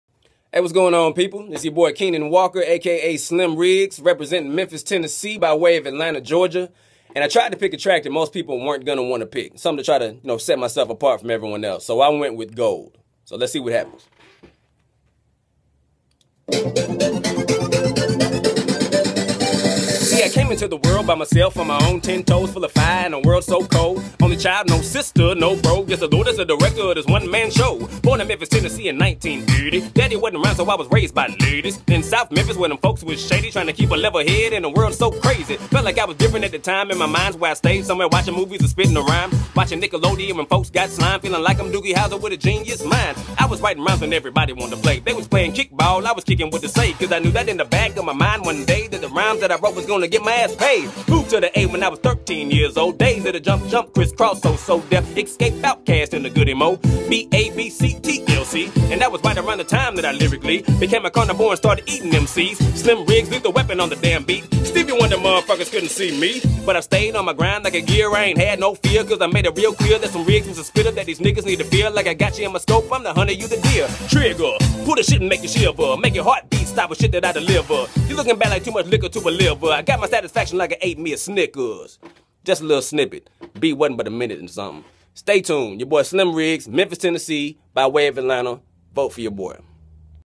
Kenon "Slim Riggs" Walker's My One Shot Hip Hop Contest Entry!!
The grand prize is $100,000 and a major record deal!! Thanks in advance for the support!!